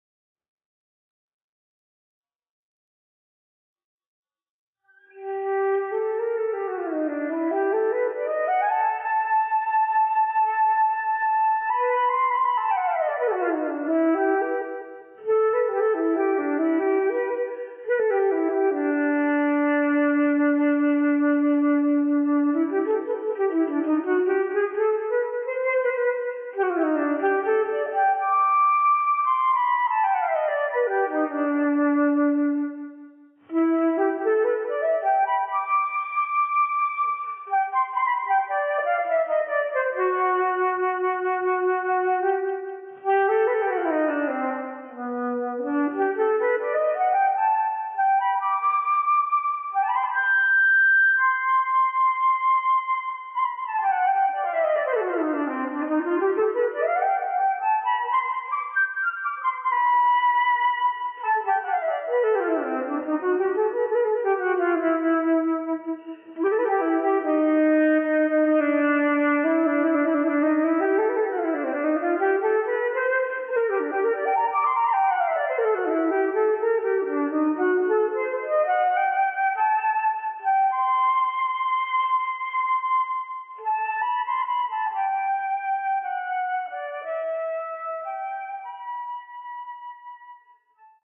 A Short Trip (Paintings Poetry Flute Improvisation English)
The Result

There was a peach tree

Tiger

They grow small flowers

Pond

Easy questions

A Short Trip

Rocks, grass and sand